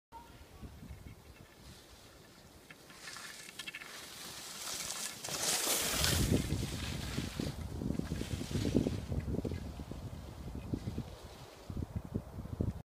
Trace: Skiing - Matt Weinstein at Sugarbush
Trace auto edited Matt Weinstein's skiing session at Sugarbush on Dec 26, 2013.

Runs: 22
Min of Skiing: 121
Vertical M: 28.2k ft
Distance: 25.5 mi
Calories Burned: 1,816
Max Speed: 39.9 mph
Max sustained speed: 37.8